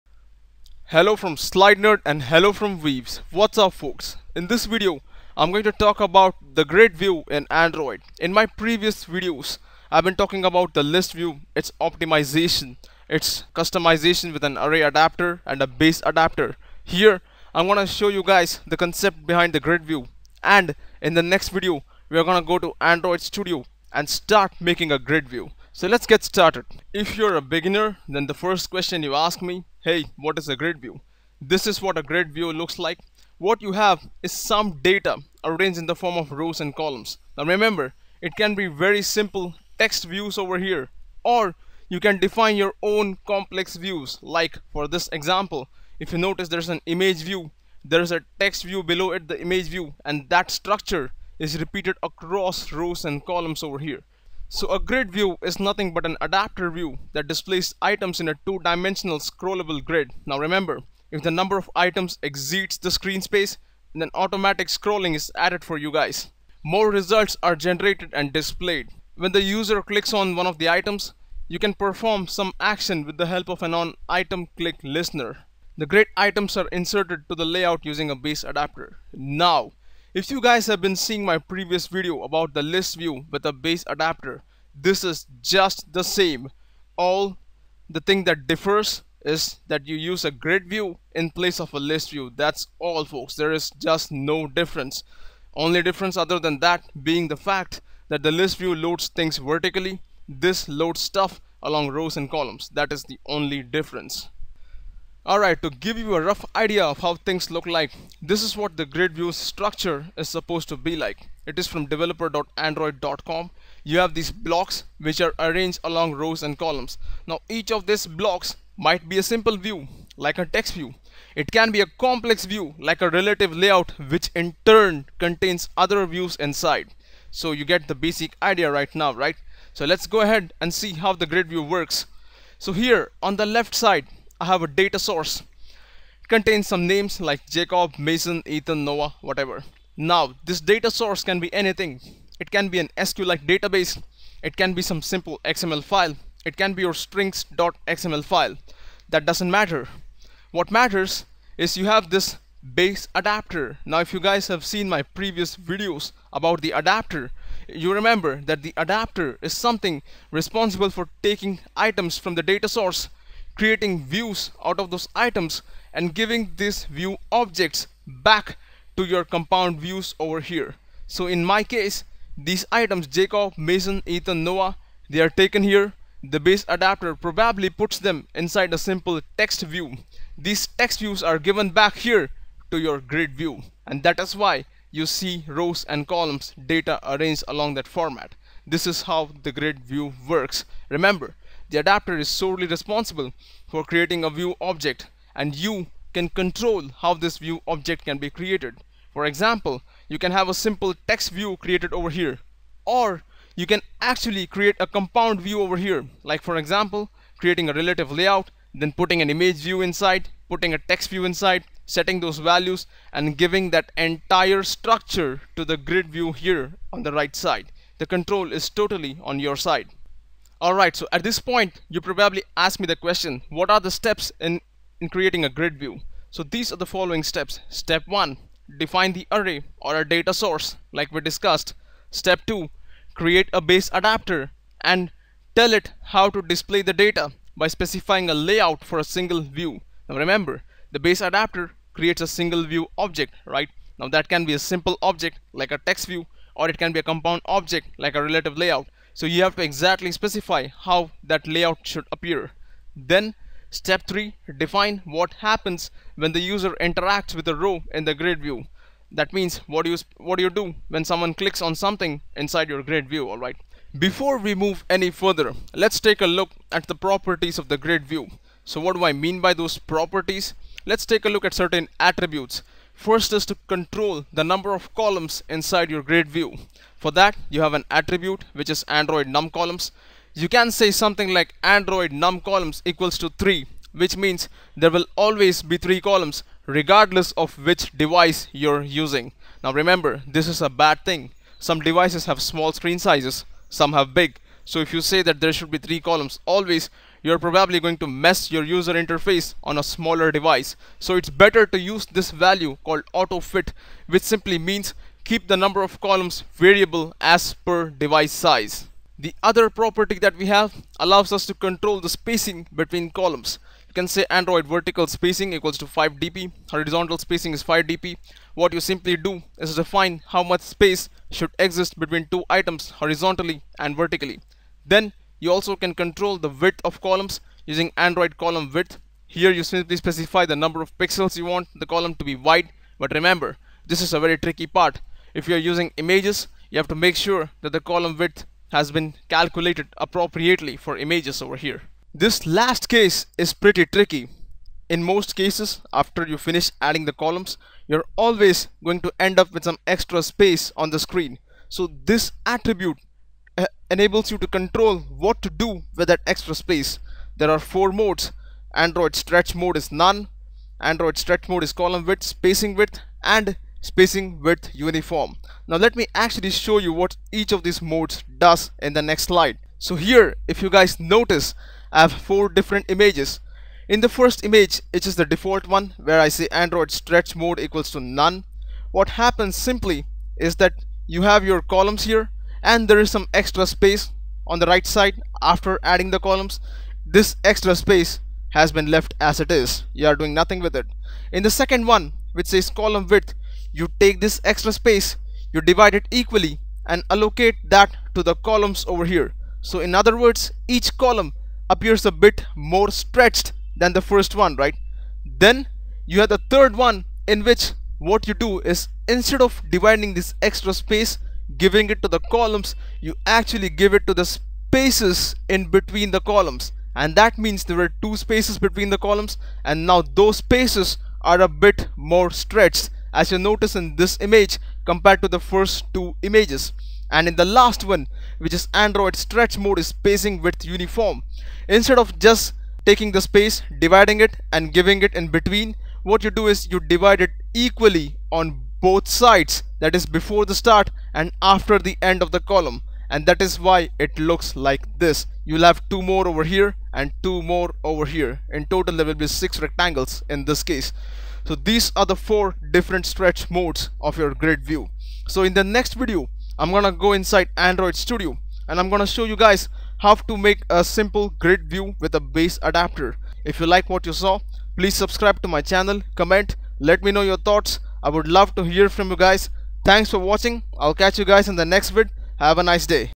97 Android GridView Tutorial Part 1
This Android GridView Tutorial video shows what is a GridView in Android, how does the GridView work as an AdapterView to accept data from an ArrayAdapter or BaseAdapter to construct View objects, how the attributes of a GridView like android:stretchMode, android:columnWidth, android:verticalSpacing, android:horizontalSpacing, android:numColumns work